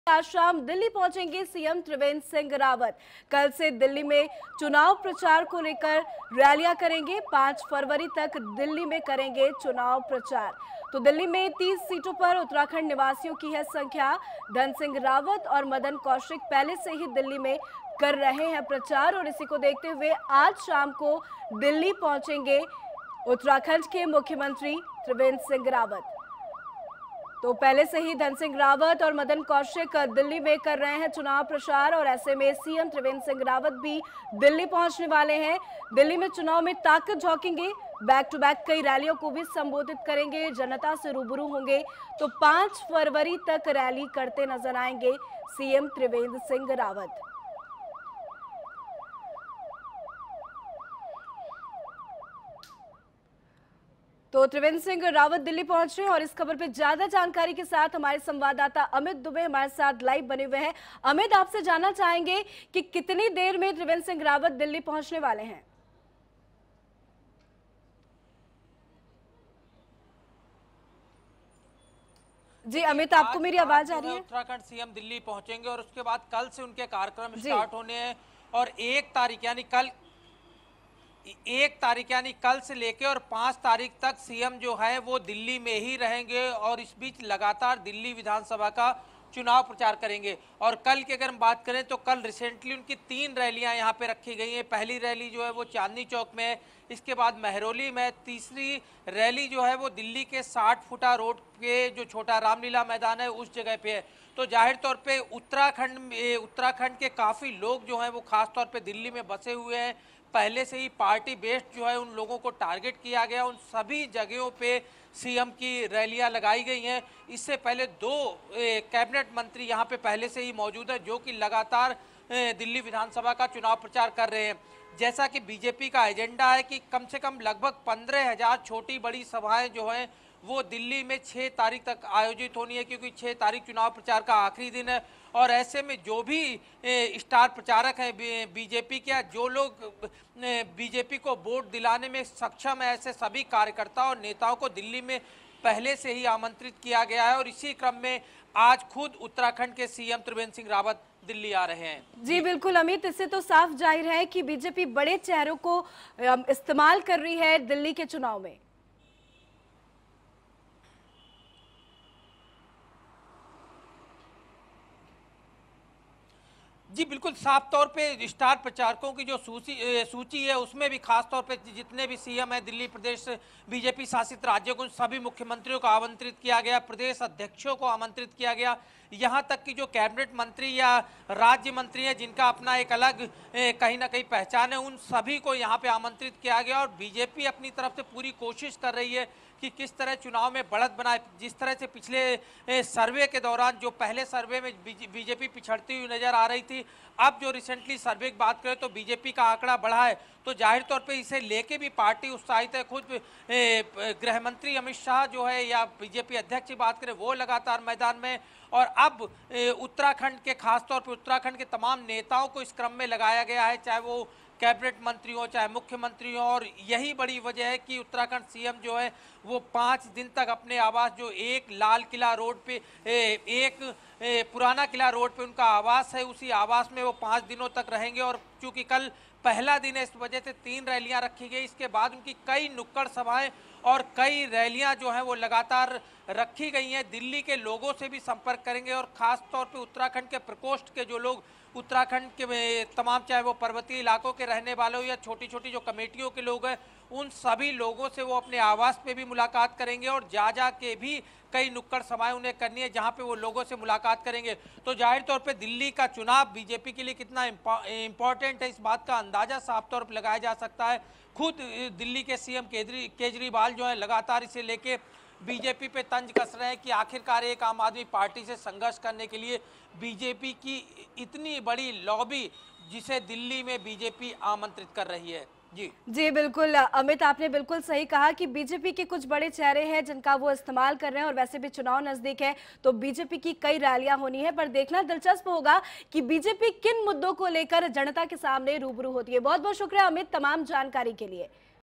सीएम त्रिवेंद्र सिंह रावत दिल्ली में करेंगे चुनाव प्रचार, कुछ ही देर में पहुंचेंगे दिल्ली | R9TV
कल से दिल्ली में चुनाव को लेकर करेंगे रैली, 5 फरवरी तक दिल्ली में करेंगे चुनाव प्रचार, दिल्ली में 30 सीटों पर उत्तराखंड निवासियों की है संख्या